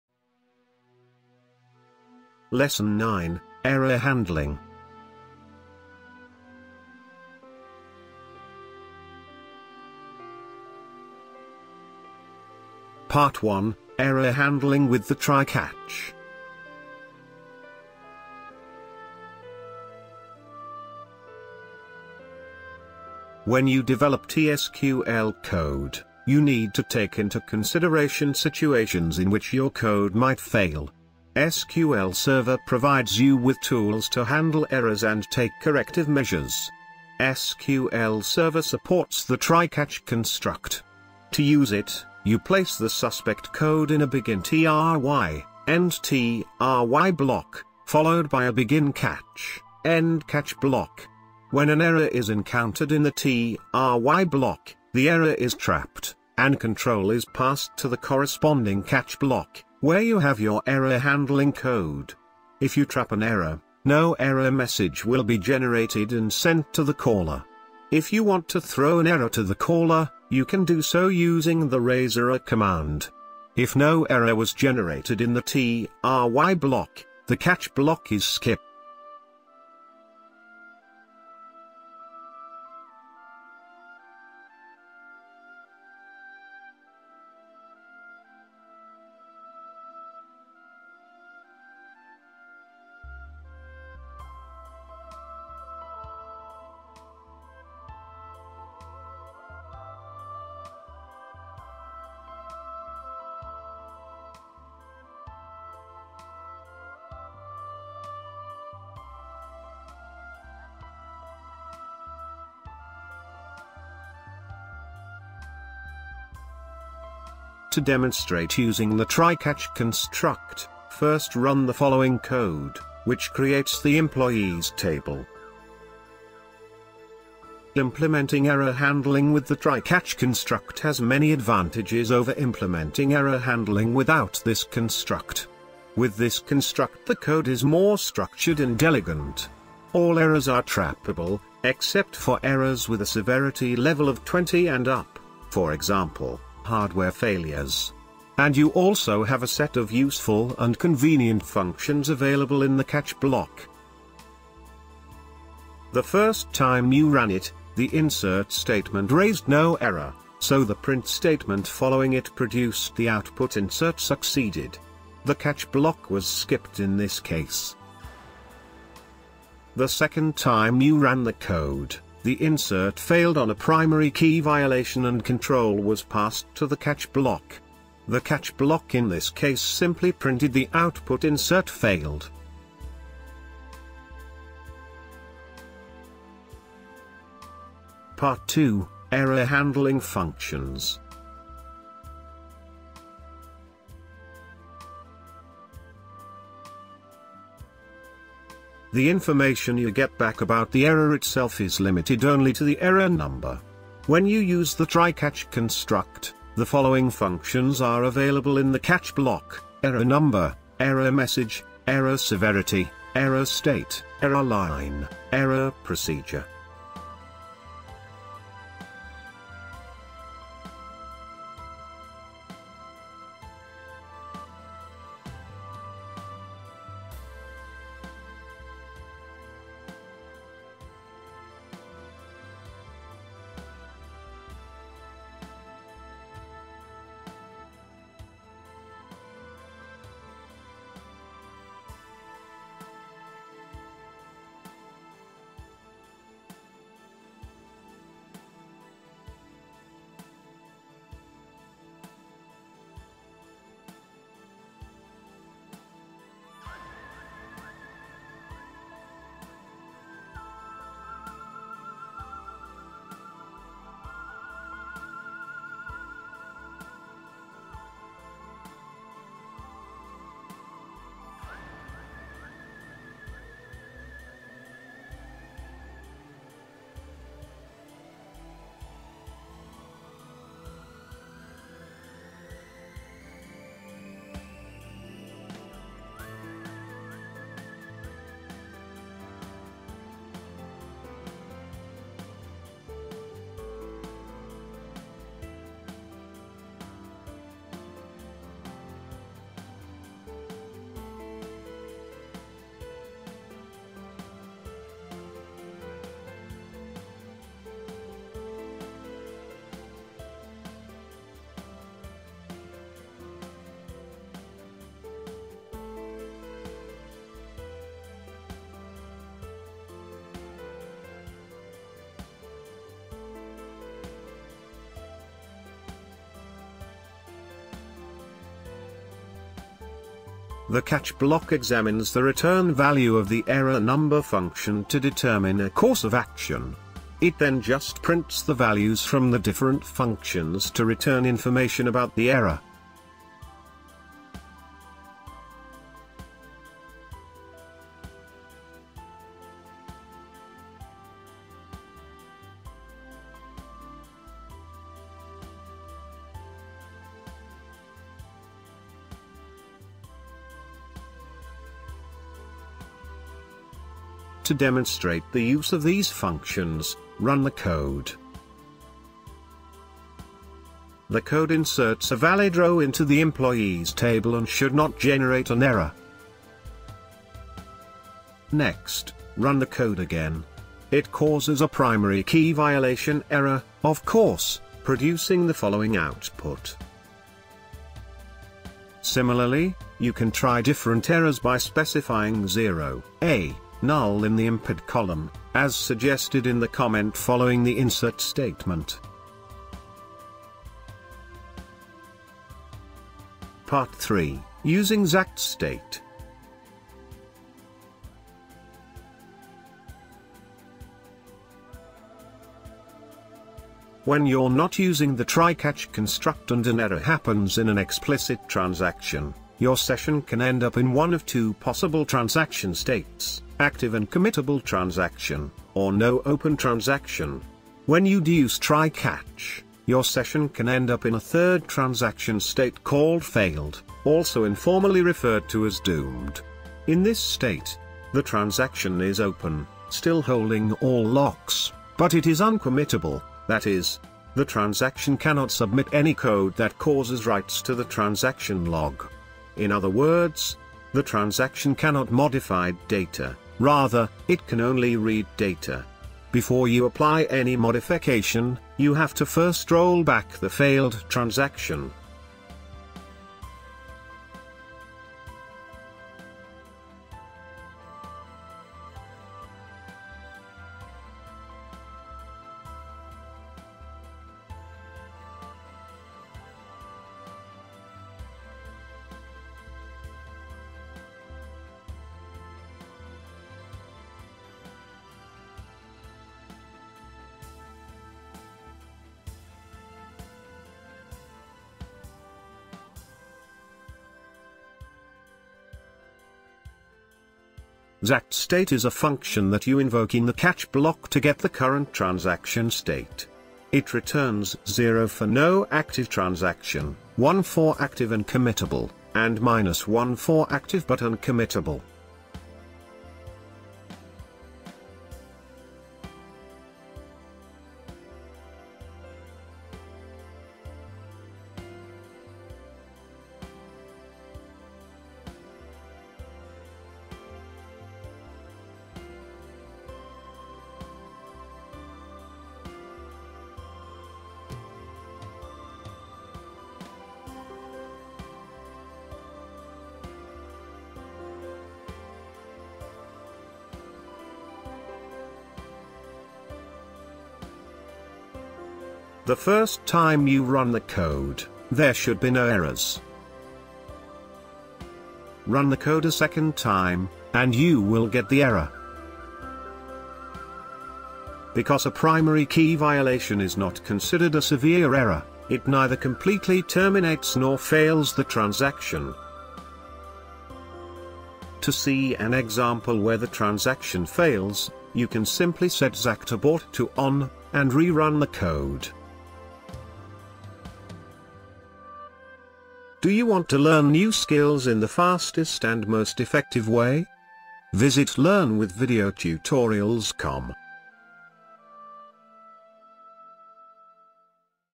T-SQL tutorial: Error Handling, Full SQL Server Transact-SQL Course, Introduction (Lesson 9)
T-SQL tutorial for beginners, SQL Server Transact-SQL Introduction.
Lesson 9:  Error Handling